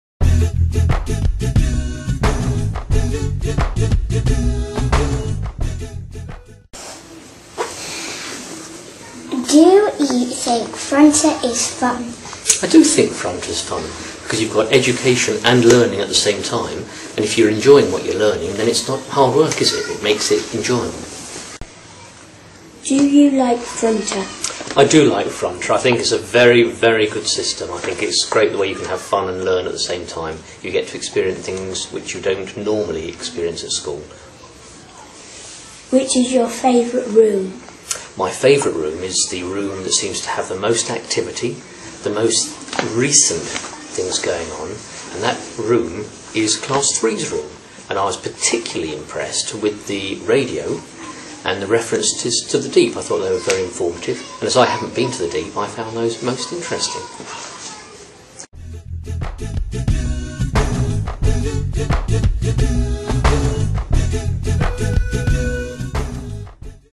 A Governor talks about Fronter.wmv
This video was made by pupils from South Otterington Primary School who asked one of their governors what he thinks of Fronter.